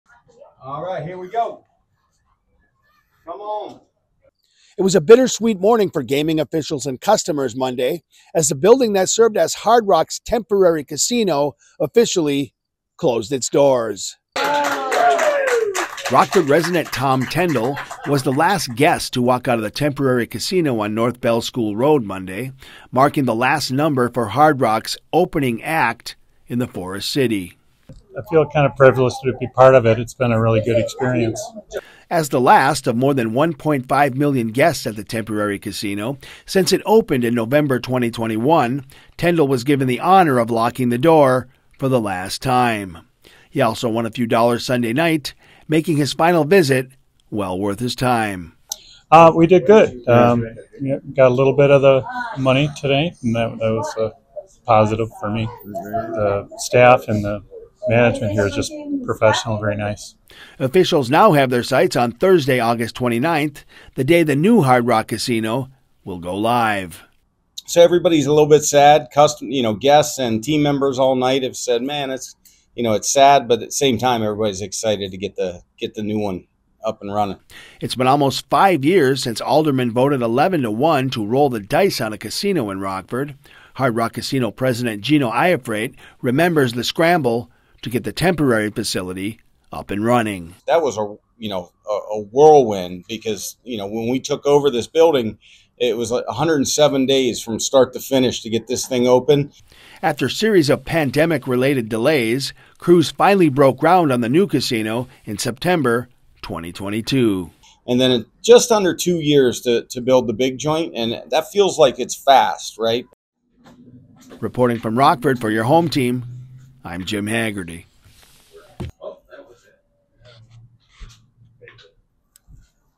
Hard Rock's "Opening Act" closes in Rockford
Rockford's temporary casino closed at 5 a.m., Monday, Aug. 12, a little more than two weeks ahead of Aug. 29, the day the new Hard Rock Casino will open at 7801 E. State St., the site of the former Clock Tower Resort.